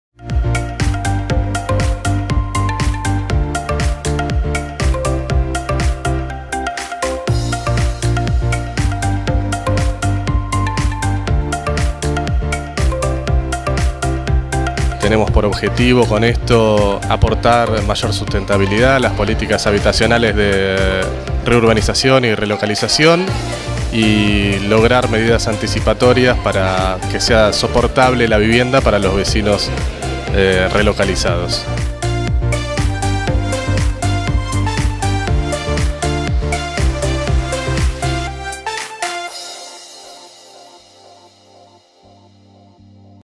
Se presentaron los resultados de la Encuesta sobre Economías Familiares
El relevamiento fue realizado por la Defensoría y el Instituto de Vivienda de la Ciudad (IVC) en los complejos habitacionales San Antonio y Santiago de Compostela.